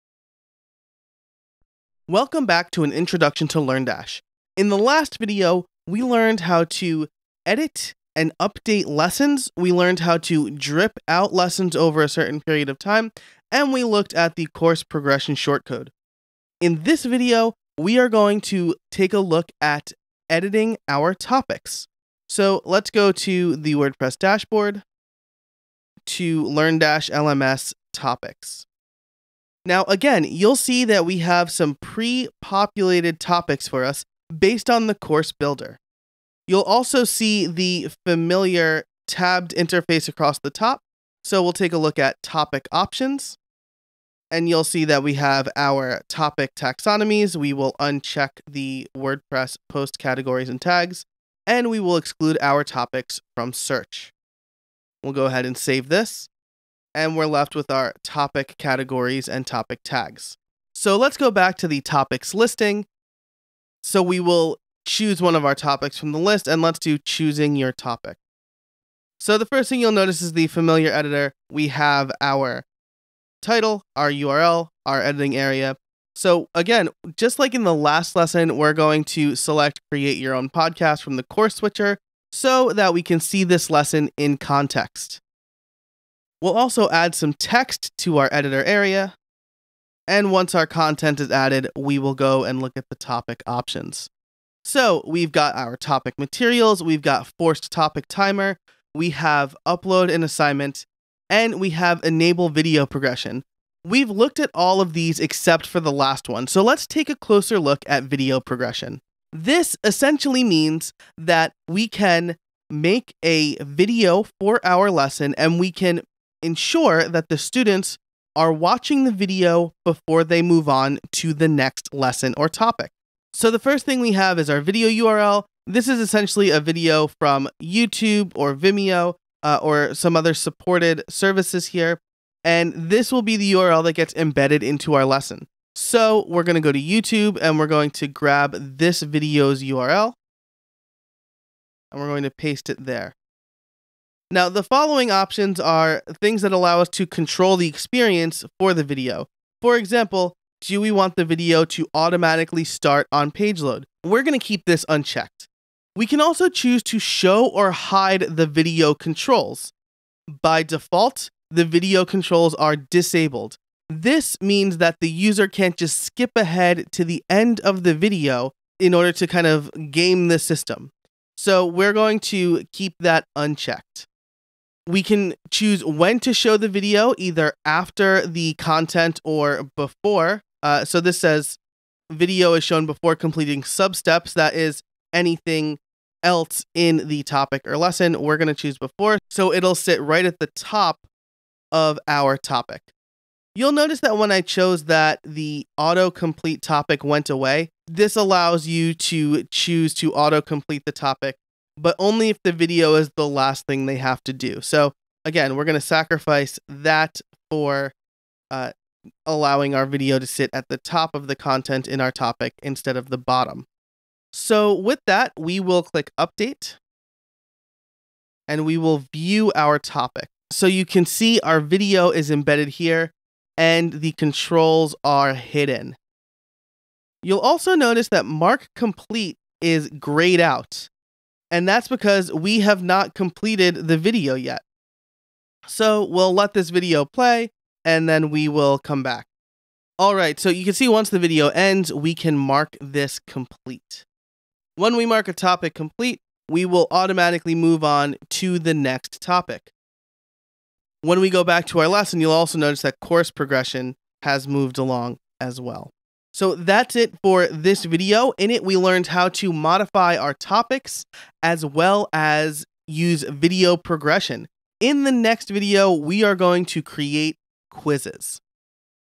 4 Adding Topics to Lessons
If you need to break-up lessons even more then you can add topics. Joe adds some topics to his own lessons.

This video series is produced by WordPress expert and trainer Joe Casabona